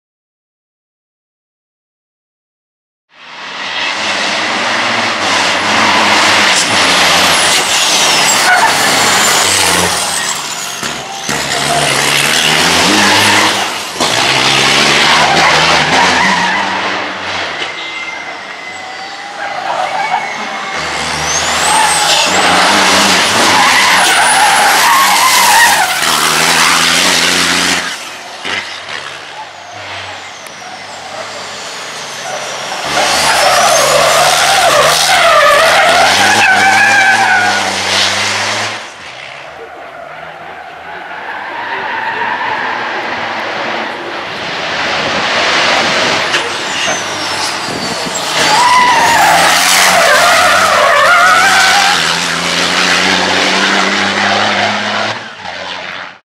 Buggyra - DRIFT - David Vršecký - Zámecký vrch 2014
Jako již každoročně, tak i letos se na Zámeckém vrchu objevil David Vršecký se svou Buggyrou